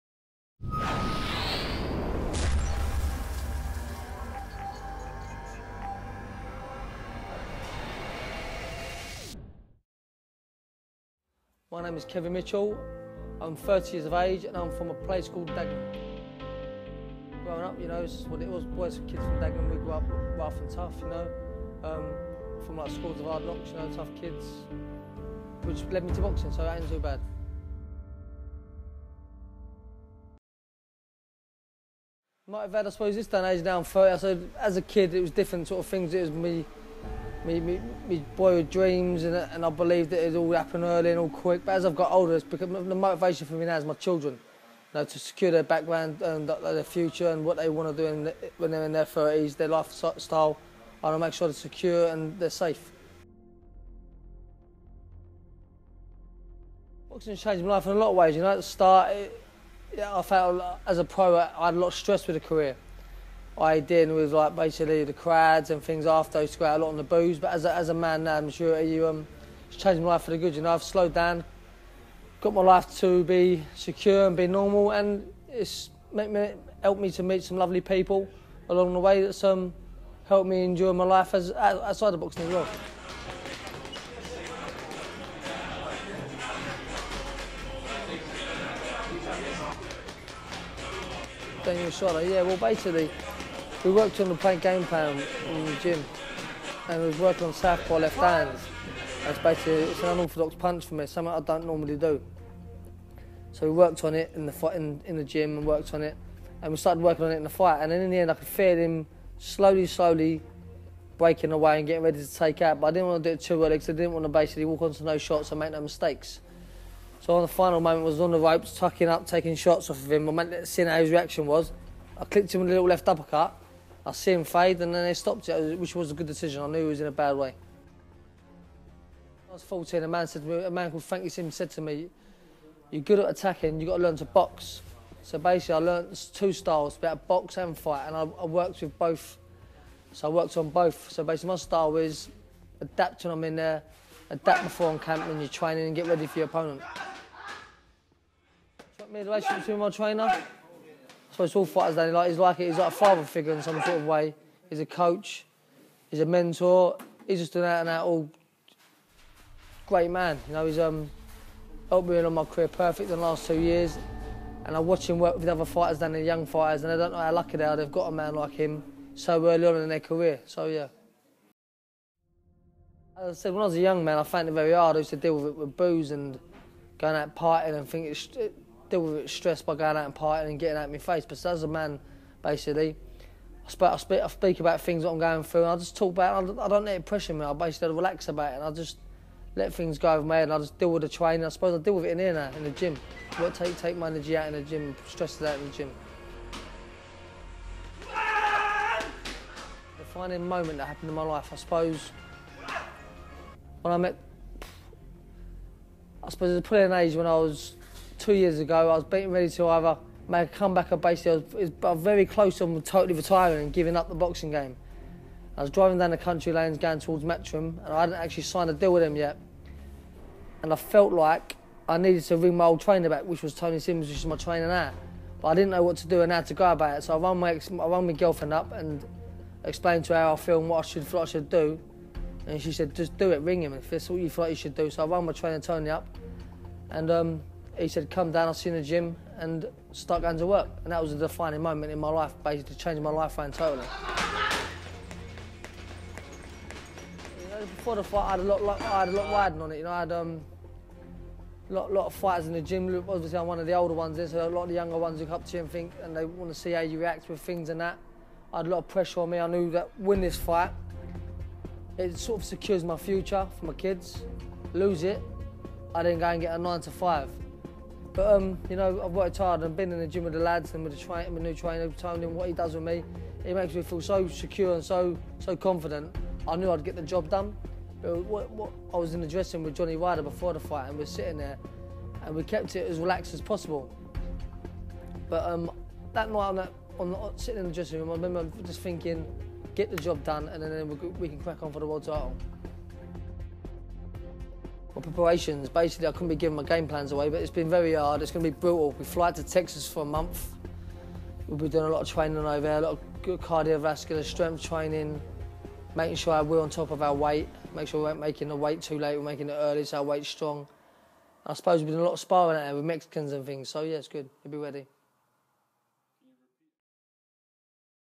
The Boxer Profile | Kevin Mitchell
Rule Britannia is only 5 days away, and we caught up with Kevin Mitchell as he prepares for his title fight May 30th at the O2 Arena in London. In this feature he discuses the defining moment in his boxing career that could of seen him retire from the sport.